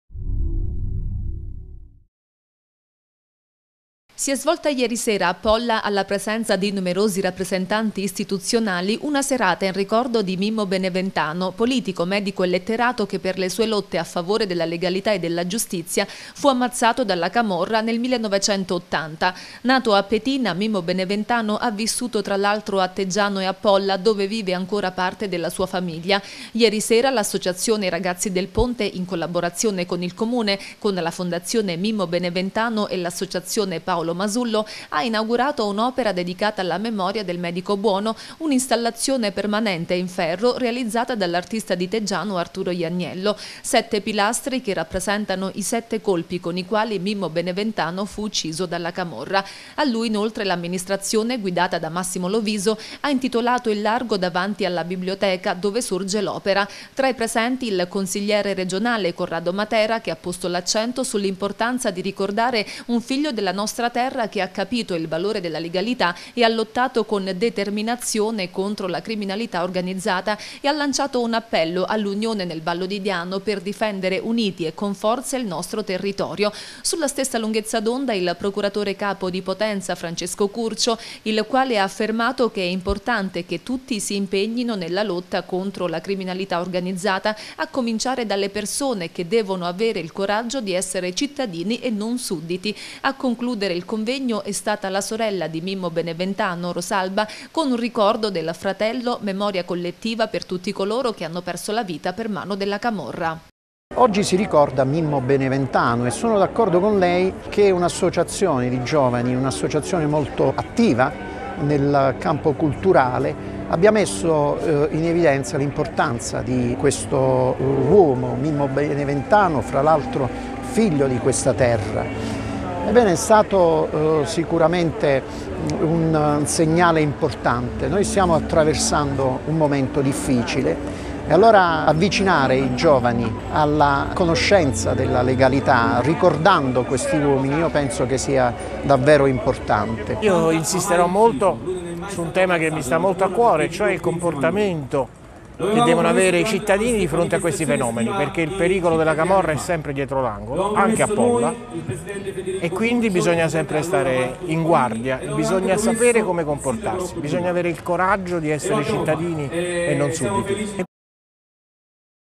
Polla, inaugurata l’opera dedicata a Mimmo Beneventano ucciso dalla Camorra
Si è svolta ieri a Polla, la cerimonia di inaugurazione dell’opera e del largo dedicati a Mimmo Beneventano. L’evento si è svolto dopo un convengo sulla legalità con numerosi rappresentanti istituzionali.